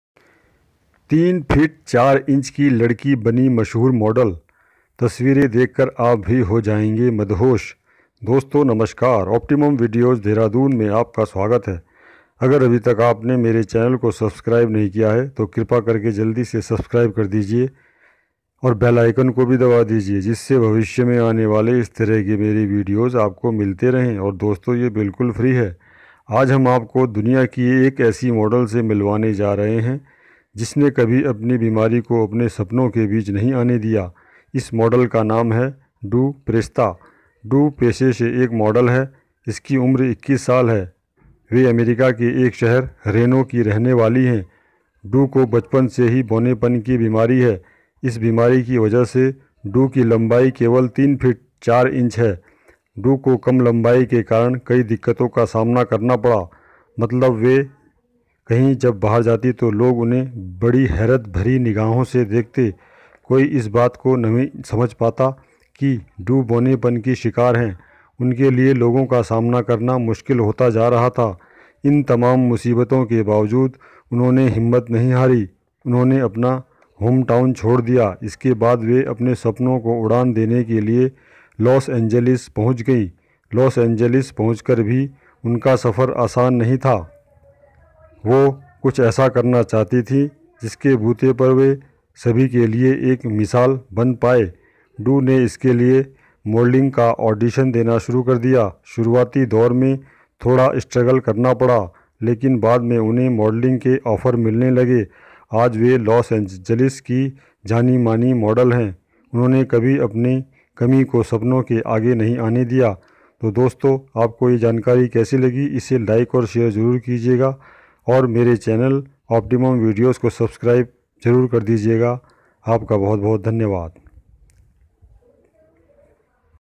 3 फीट 4 इंच की लड़की बनी मशहूर मॉडल, तस्वीरें देखकर आप भी हो जायेंगे मदहोश//small girl,hot modal.
अपने बौनेपन की बीमारी के बावजूद इस लड़की ने मॉडलिंग की दुनियां में जो नाम कमाया वो काबिले तारीफ है ।